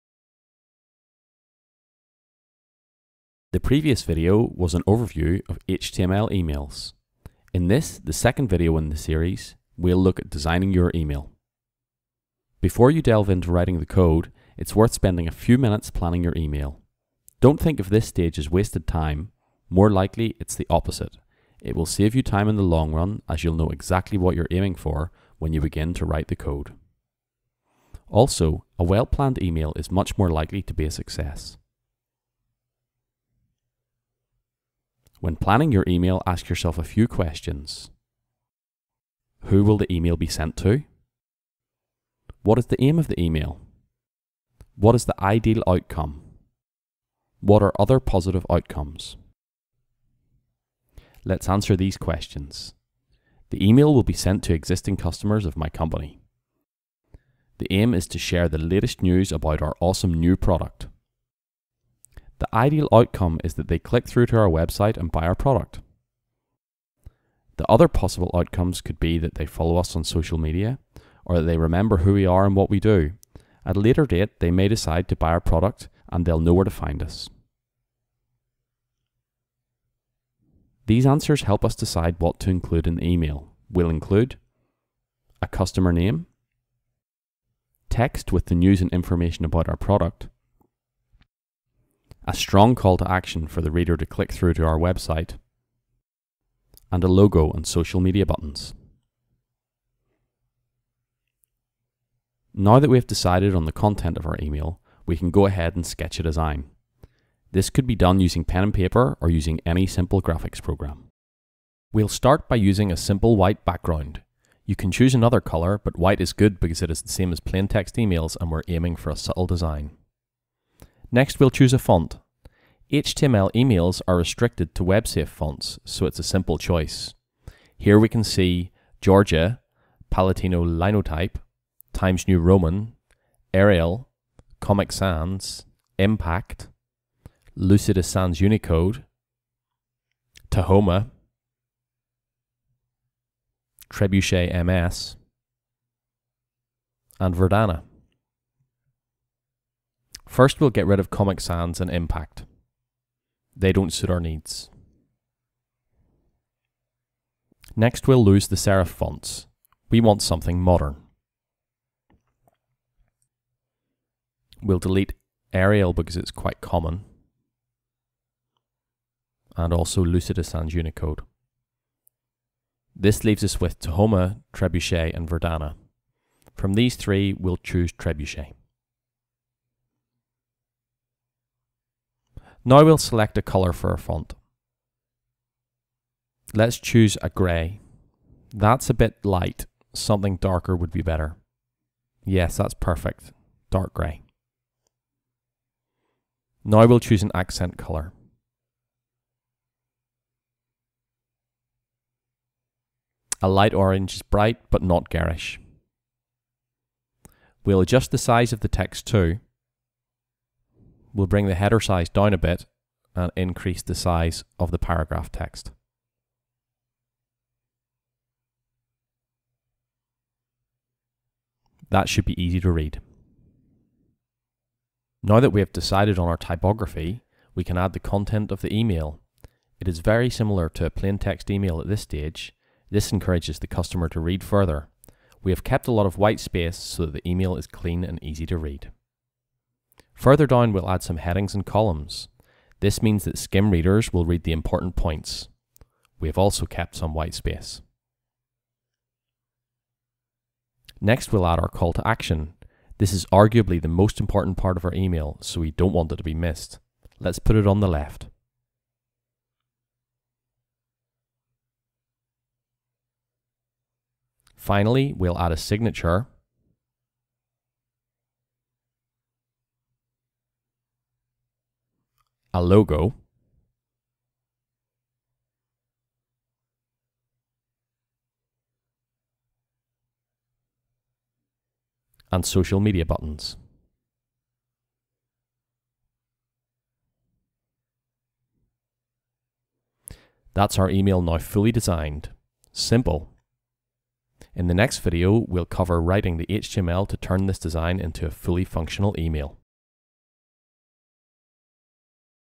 How To Create HTML Emails - Part2: Design
Designing an HTML email is an important step which will save you time when you come to write the HTML. See how to design your email in this short video.